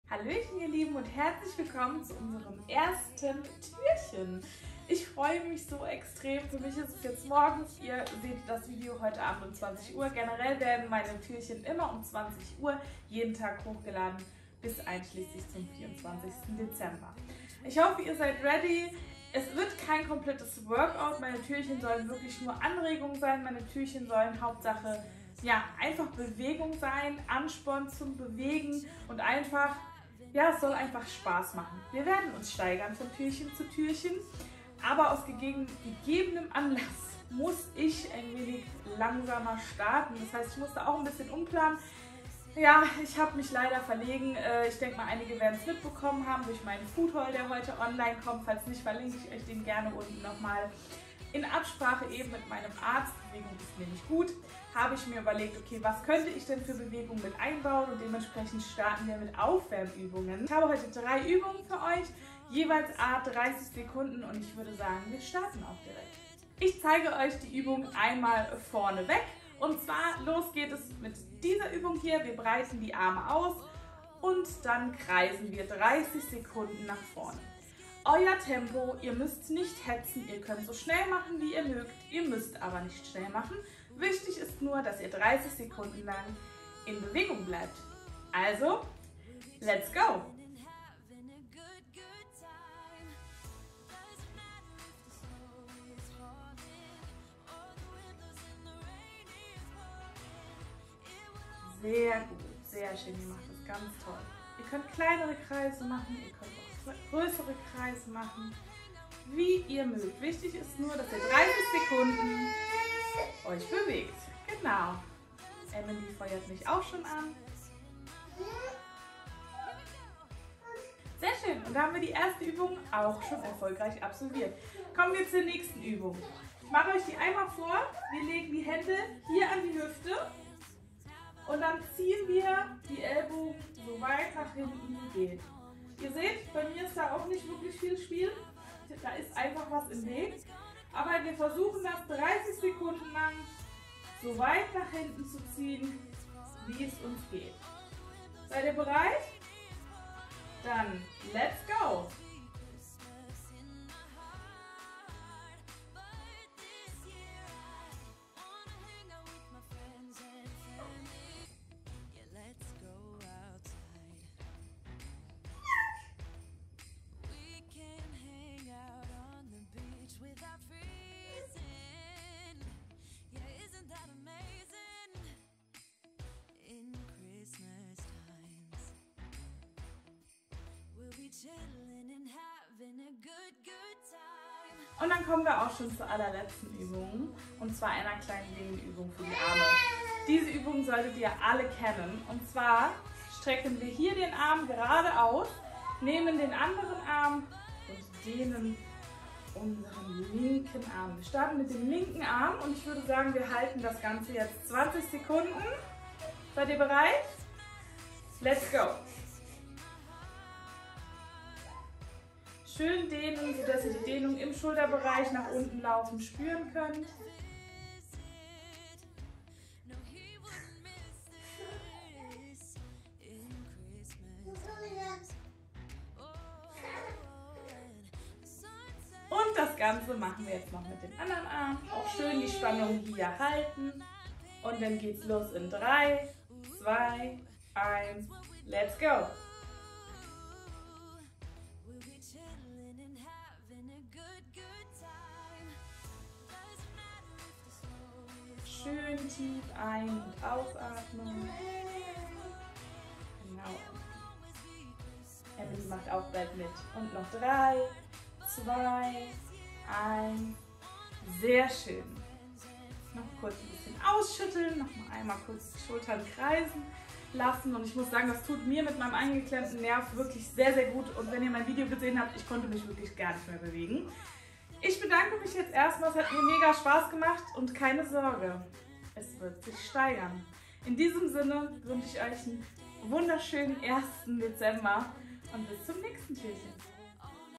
1. Adventstürchen ..Es kann losgehen Verlosung21
schön das ihr mit dabei seit:)
Ich hoffe ihr seit gut in den Dezember und die damit verbundene vorweihnachtliche Zeit gestartet.
Für mich geht es jetzt nach meiner etwas stressigeren Zeit endlich zur besinnlicheren Zeit über und ich freue mich schon sehr darauf.
Ich hoffe euch gefällt das erste Adventskalendertürchen.
Falls ihr an der Verlosung teilnehmen möchtet, dann sag ich euch gleich nochmal wie das geht:)
Falls ihr wissen wollt worum es nochmal geht und ihr mein Video dazu verpasst habt , dann schaut euch das Video gerne noch an.

Über ein Abo von euch würde ich mich sehr freuen.

Zur Verlosung:

Um an der Verlosung teilzunehmen :
Musst du über 18 sein.
Unter 1 oder alle 24 Türchen ein Kommentar mit dem Hashtag Verlosung21 dalassen sowie ein Däumchen nach oben.
Es steht dir frei dies unter jedem Türchen zu machen oder vllt auch nur unter deiner persönlichen Glückszahl.
Am 24.12.21 werde ich unter allen Türchen 2 Videos auswählen und unter den dort vorhandenen Kommentaren auslosen.

Ich freue mich sehr über eure Kommentare und bin schon sehr gespannt wie euch der Adventskalender gefällt.

Ich wünsche euch besinnliche Adventstage.

Eure
Embellie